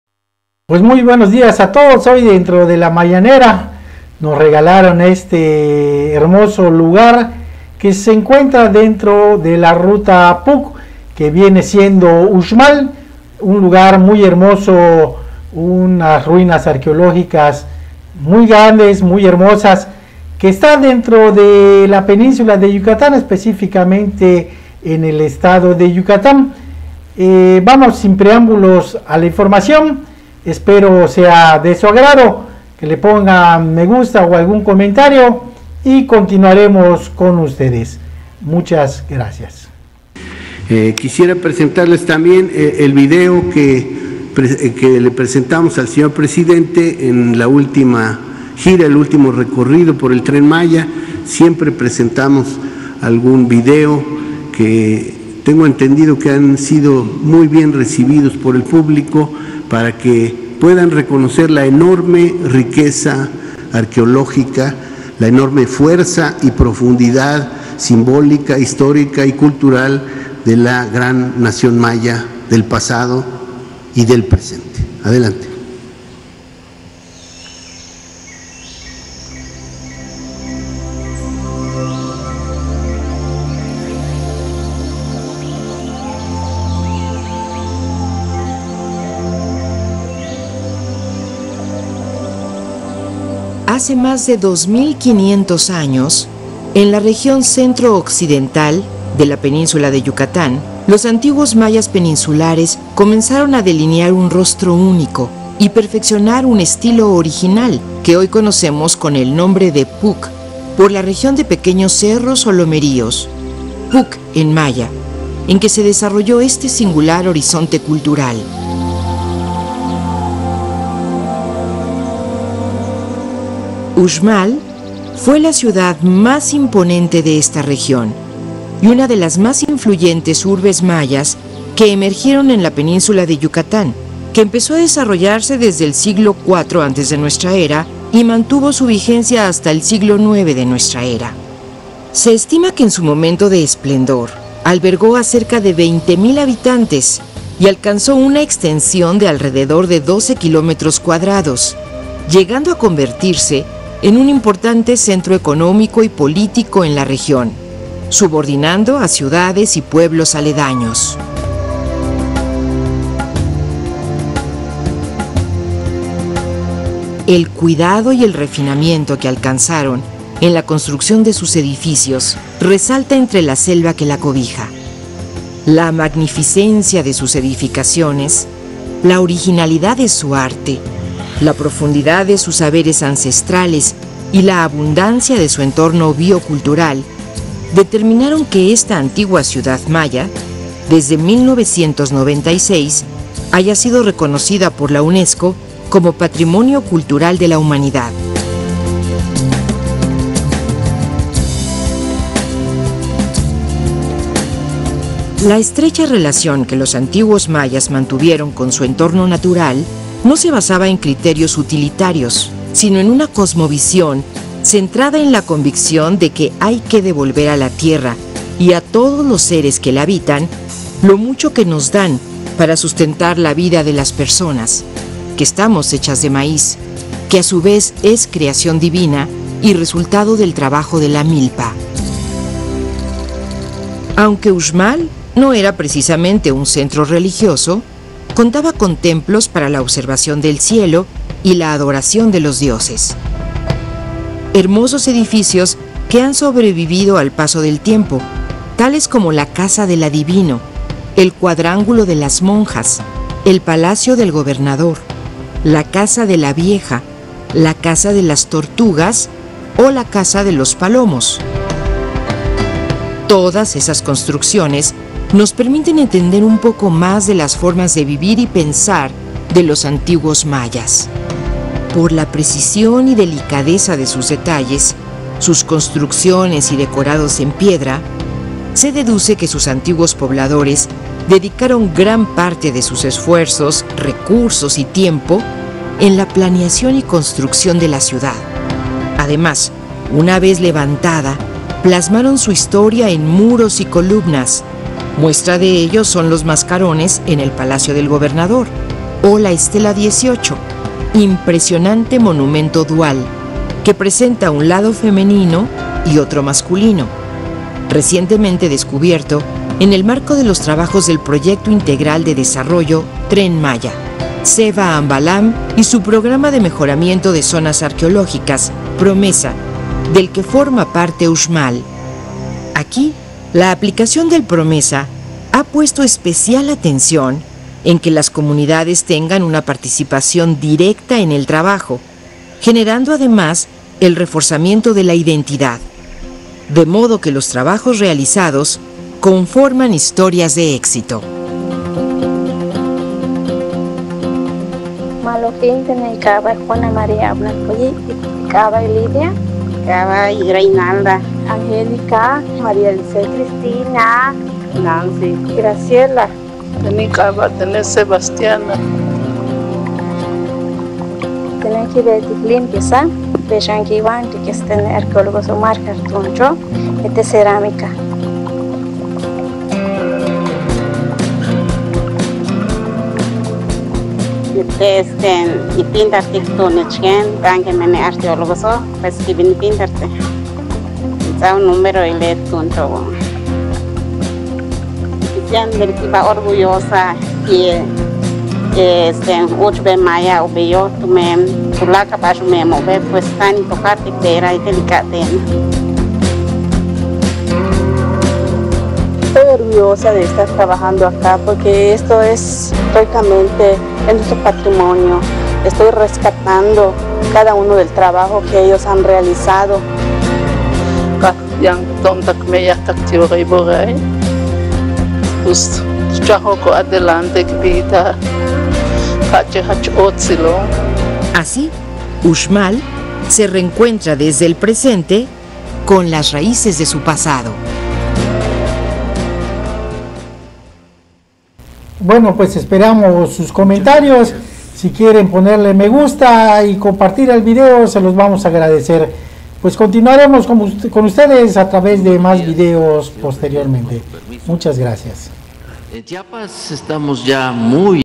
Uxmal, esplendoroso lugar dentro de la Ruta Puc y la del Tren Maya
De la rueda de prensa mañanera del dìa de hoy y un vìdeo puesto ya hace unos días, iremos hasta Uxmal que fue la ciudad más imponente de la región centro occidental de la Península de Yucatán, y una de las más influyentes urbes mayas que emergieron en  esta zona; la cual comenzó a desarrollarse desde el siglo IV antes de nuestra era, y mantuvo su vigencia hasta el siglo IX de nuestra era.
El proyecto integral de desarrollo Tren Maya, Séeba’an báalam y su Programa de Mejoramiento de Zonas Arqueológicas, Promeza, ha puesto especial atención en que las comunidades tengan una participación directa en el trabajo, generando además el reforzamiento de la identidad.

Haz un comentario